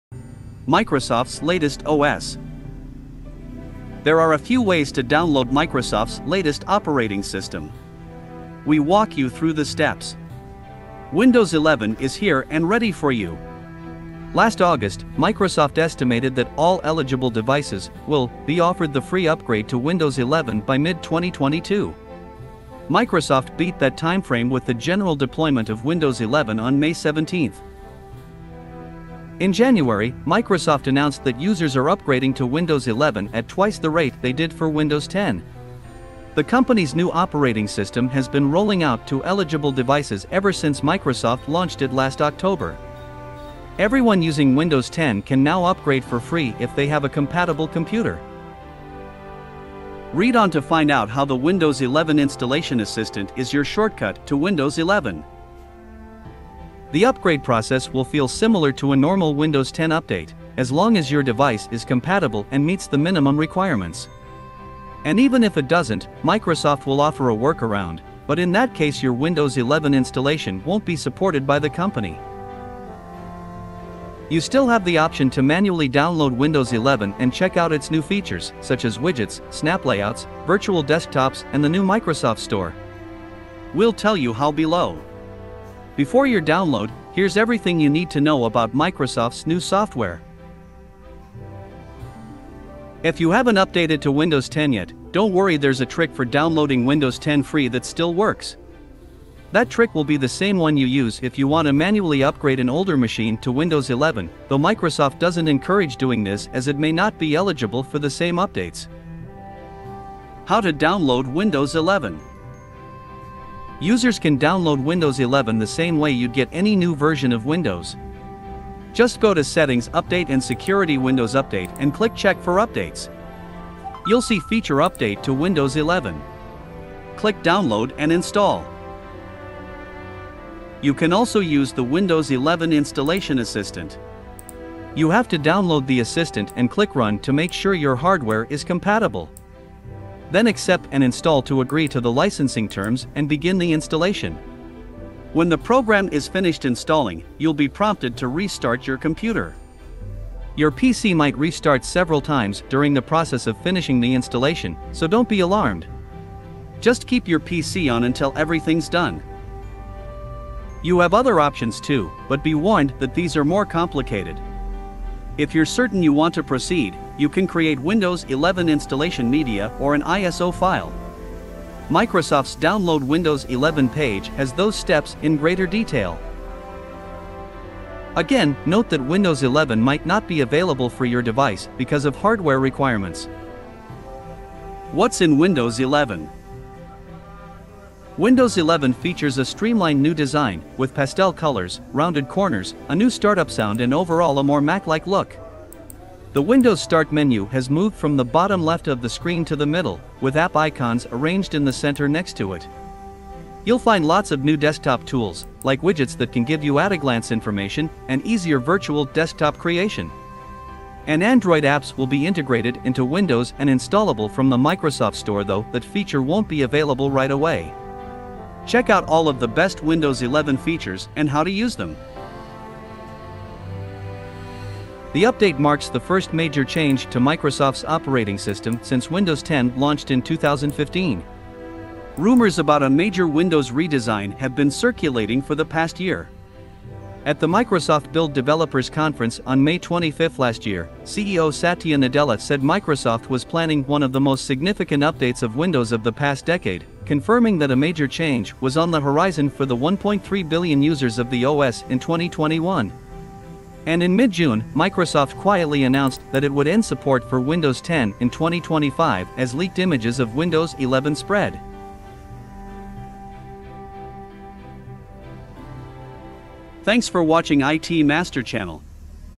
Windows 11 How to Download Microsoft's Latest OS
@windows 11 @itmasterurl
There are a few ways to download Microsoft's latest
operating system. We walk you through the steps. Windows
11 is here and ready for you. Last August, Microsoft estimated that "all
eligible devices [will] be offered the free upgrade to Windows 11 by
mid-2022." Microsoft beat that timeframe with the general deployment
of Windows 11 on May 17.
In January, Microsoft announced that users are upgrading to Windows
11 at twice the rate they did for Windows 10. The company's new
operating system has been rolling out to eligible devices ever since
Microsoft launched it last October. Everyone using Windows 10 can
now upgrade for free -- if they have a compatible computer.
Read on to find out how the Windows 11 Installation Assistant is your
shortcut to Windows 11.
The upgrade process will feel similar to a normal Windows 10 update,
as long as your device is compatible and meets the minimum
requirements. (And even if it doesn't, Microsoft will offer a workaround,
but in that case your Windows 11 installation won't be supported by